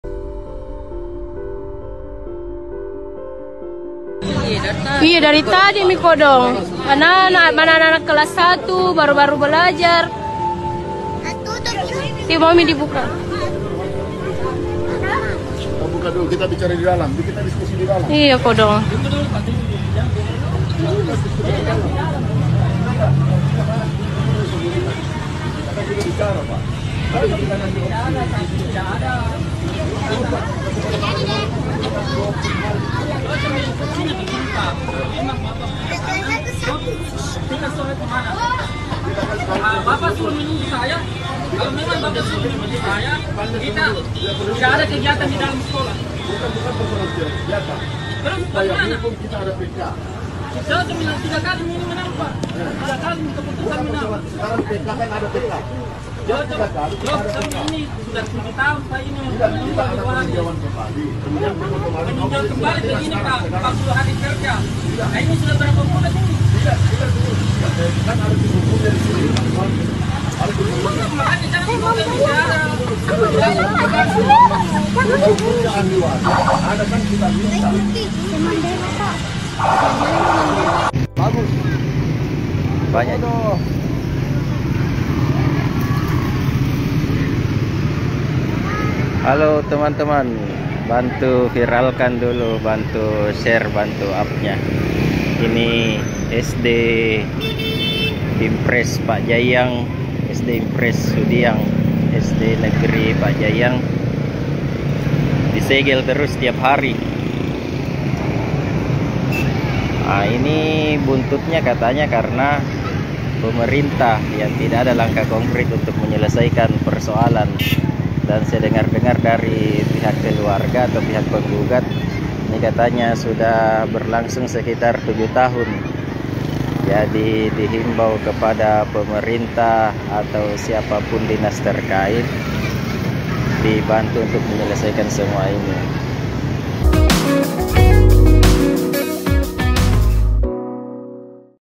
SD Inpres Pajjaiang Makassar Kembali Disegel, Puluhan Murid Terlantar
SD Inpres Pajjaiang yang terletak di Kecamatan Biringkanaya, Makassar, kembali disegel, Rabu (17/7/2024).

Video terkini dan terbaru..